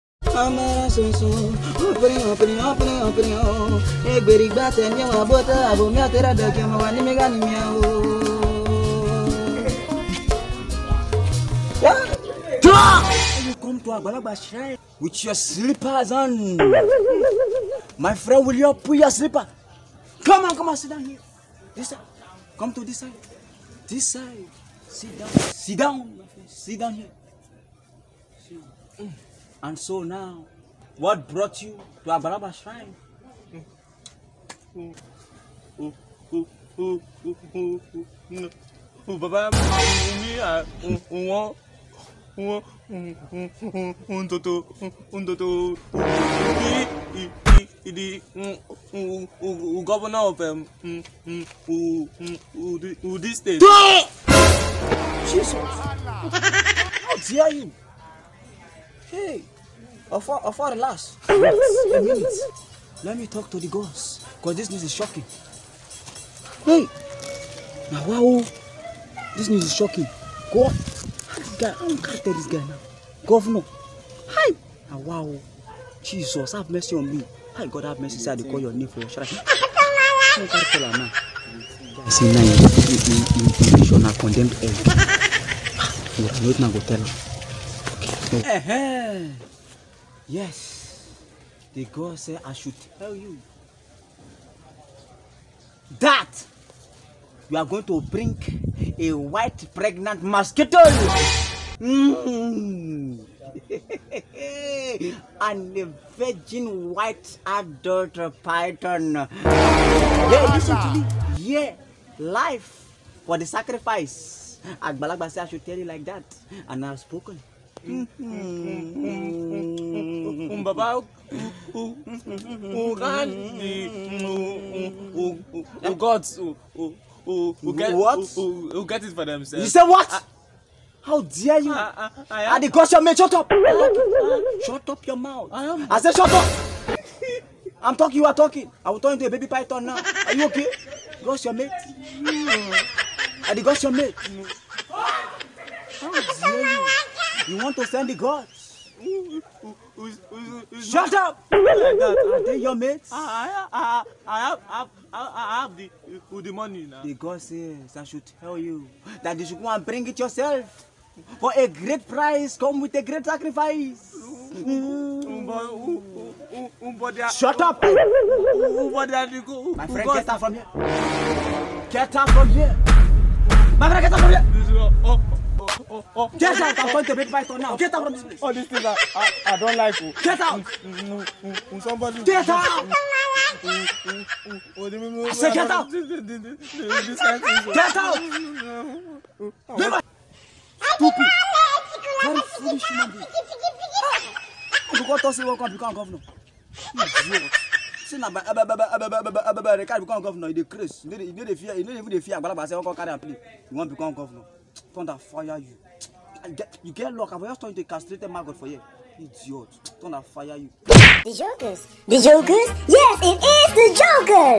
Stammering Palava 3 by { d jokerscomedy}
A Stammering man when to a native doctor requesting what was impossible. want to what it is?